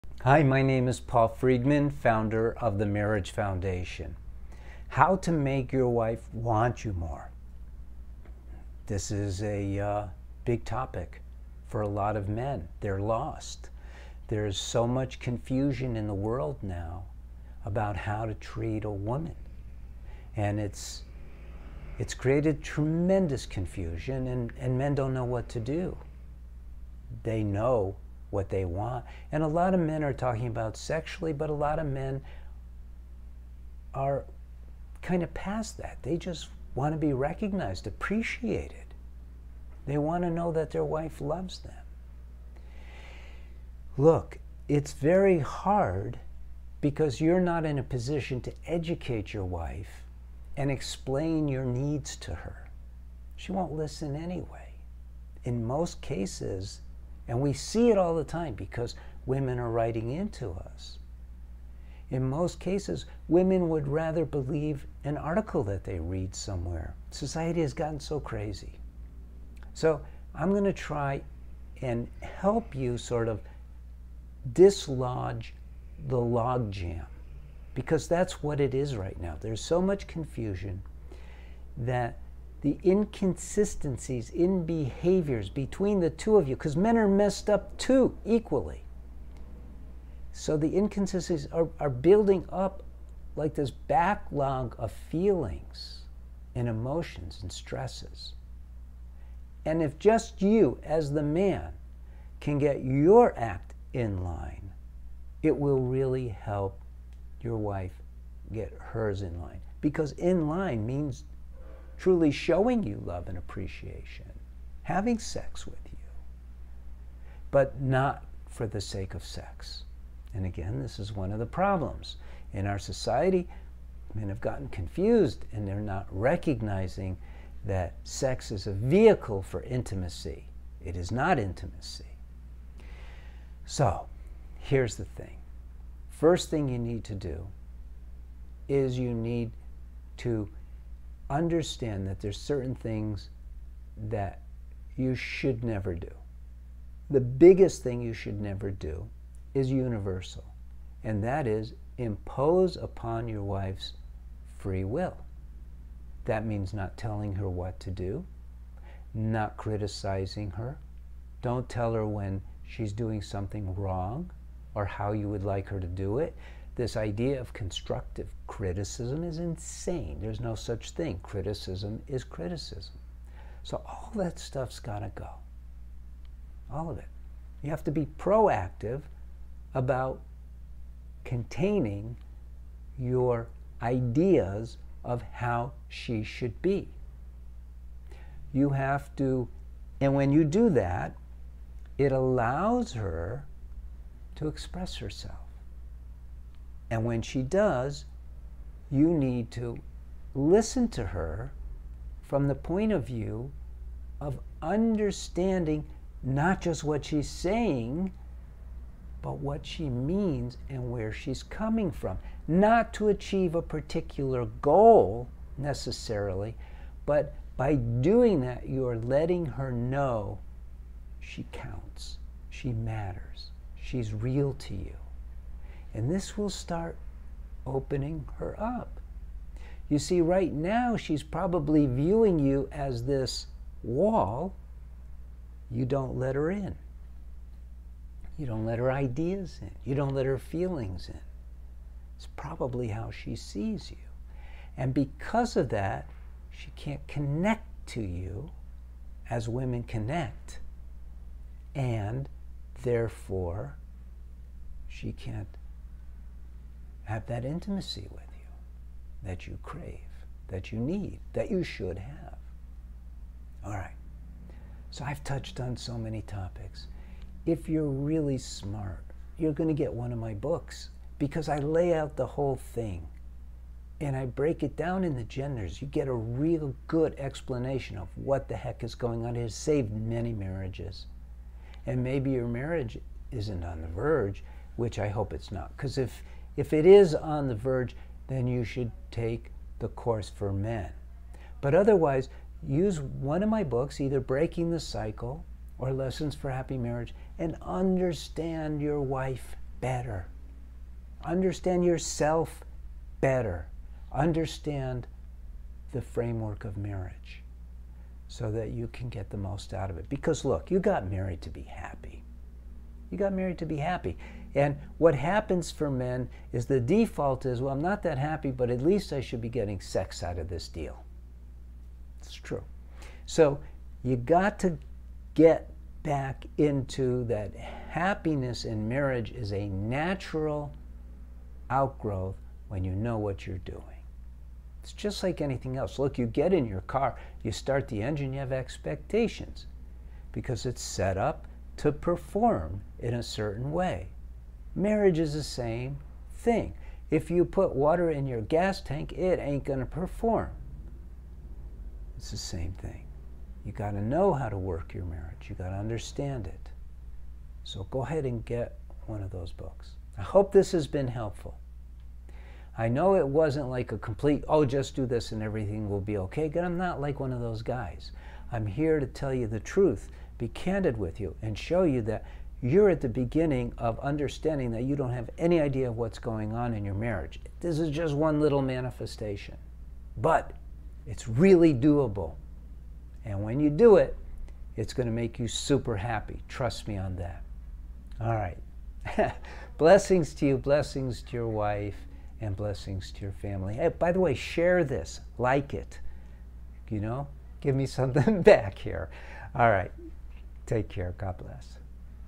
How To Make Your Wife Want You More | Paul Friedman
Are you thinking on how to make your wife want you more? Ways to make your spouse want you more? This video is ideal for giving you the kind of insights that will help you set a better path for you to take, than just accepting the inevitable.

This is a big topic for a lot of men -- they're lost. There is so much confusion in the world now about how to treat a woman and it's created tremendous confusion and men don't know what to do. They know what they want and a lot of men are talking about sex, but a lot of men are kind of past that. They just want to be recognized, appreciate it. They want to know that their wife loves them.

It's very hard because you're not in a position to educate your wife and explain your needs to her -- she won't listen anyway. In most cases, women would rather believe an article that they read somewhere, society has gotten so crazy. I'm going to try and help you sort of dislodging the log jam because that's what it is right now.

There's so much confusion that the inconsistencies in behaviors between the two of you because men are messed up too equally so the inconsistencies are building up like this backlog of feelings and emotions and stresses, and if just you as the man can get your act inline it will really help your wife get hers in line because inline means truly showing you love and appreciation, having sex with you but not for the sake of sex.

This is one of the problems in our society. Man has gotten confused and they're not recognizing that sex is a vehicle for intimacy, it is not intimacy. Here's the thing, first thing you need to do is you need to understand that there are certain things that you should never do. The biggest thing you should never do is universal and that is -- impose upon your wife's free will that means not telling her what to do, not criticizing her, don't tell her when she's doing something wrong or how you would like her to do it.

This idea of constructive criticism is insane there's no such thing. Criticism is criticism so all that stuff's gotta go, all of it. You have to be proactive about containing your ideas of how she should be. You have to and when you do that it allows her to express herself and when she does, you need to listen to her from the point of view of understanding not just what she's saying but what she means and where she's coming from. Not to achieve a particular goal necessarily but by doing that you are letting her know she counts, she matters, she's real to you and this will start opening her up.

Right now, she's probably viewing you like this wall -- you don't let her in. You don't let her ideas and you don't let her feelings and it's probably how she sees you and because of that, she can't connect to you as women connect and therefore, she can't have that intimacy with you that you crave, that you need, that you should have.

I've touched on so many topics. If you're really smart, you're going to get one of my books because I lay out the whole thing and I break it down in the genders. You get a really good explanation of what the heck is going on. It saved many marriages and maybe your marriage isn't on the verge which I hope it's not because if it is on the verge then you should take the course for men but otherwise, use one of my books either Breaking the Cycle or Lessons for Happy Marriage and understand your wife better, understand yourself better, understand the framework of marriage so that you can get the most out of it.

You got married to be happy and what happens for men is the default is, "Well, I'm not that happy but at least I should be getting sex out of this deal," that's true. You got to get back into that happiness and marriage is a natural outgrowth when you know what you're doing. It's just like anything else.

You get in your car, you start the engine. You have expectations because it's set up to perform in a certain way. Marriage is the same thing. If you put water in your gas tank it ain't going to perform, it's the same thing. You got to know how to work your marriage. You got to understand it. Go ahead and get one of those books.

I hope this has been helpful. I know it wasn't like a complete, "Oh, just do this and everything will be okay." I'm not like one of those guys. I'm here to tell you the truth. Be candid with you and show you that you're at the beginning of understanding that you don't have any idea of what's going on in your marriage. This is just one little manifestation but it's really doable, and when you do it it's going to make you super happy, trust me on that.